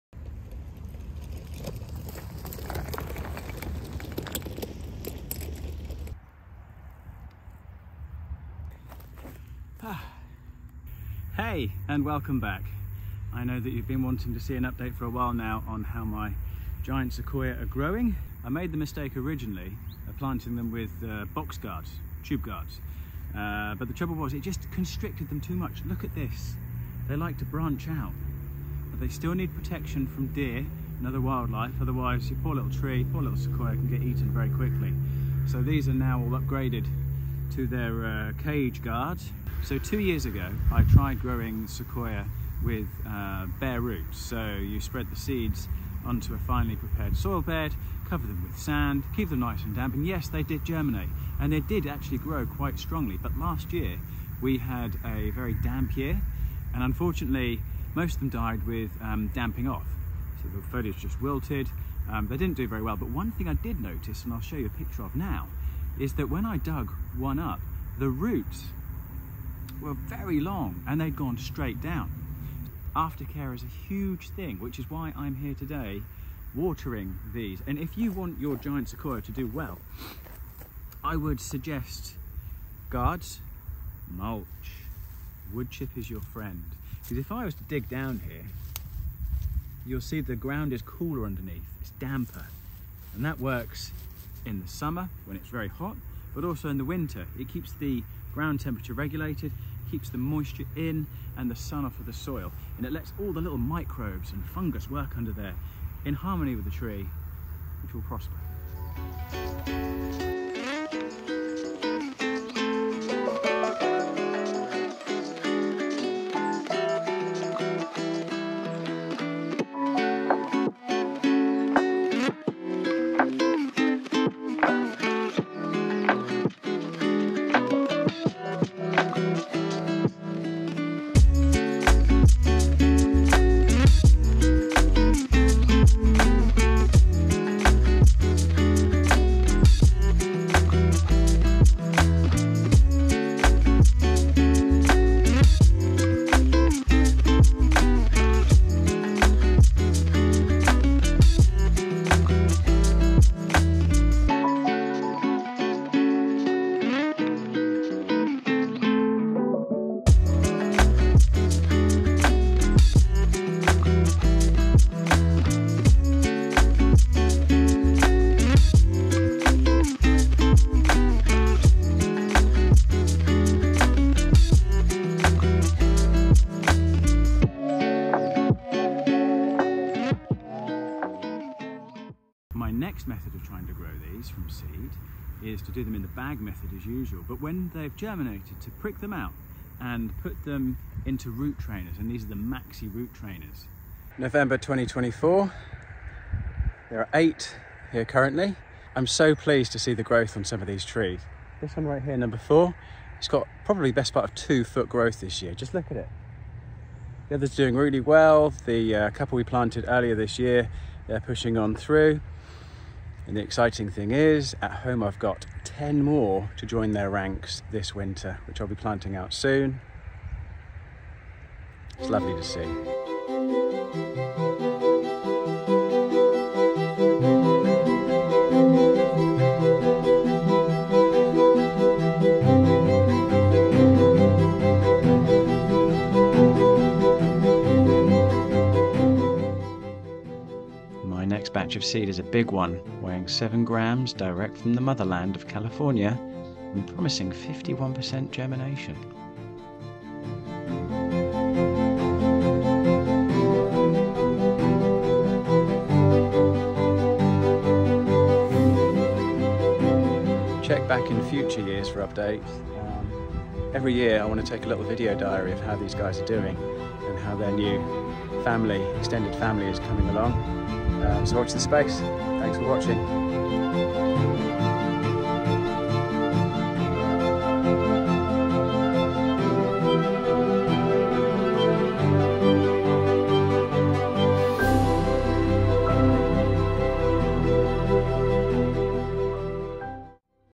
Giant Sequoia 2024 update
In 2015 I started growing sequoiadendron, and in 2020 I planted out the first three.  In spring 2024 I planted another 5 and we return in October to see their progress.

This November I will plant out another ten and have started the giant batch with 1,200 seeds bought from Sheffields Seeds promising 51% germination (let's see!).

If you want to grow your own, you must check out George Maier and William Jin's work at Superlative Trees.   They have trialled many conditions to maximise sequoiadendron germination, and it is this technique I use now.  They also have YT how-to videos.

Check back in future years as the Redwood Grove continues to grow and spread.